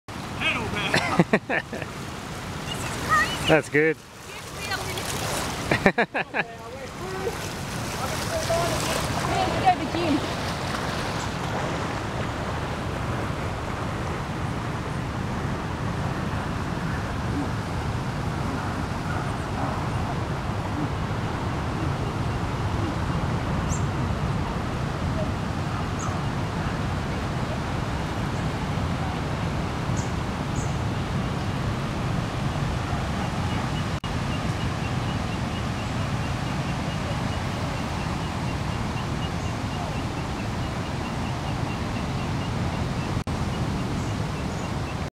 Barrack Point Floods - Surfrider Under Water
Water water everywhere ! Surfrider Caravan Park in Barrack Point becomes part of lake for a few hours.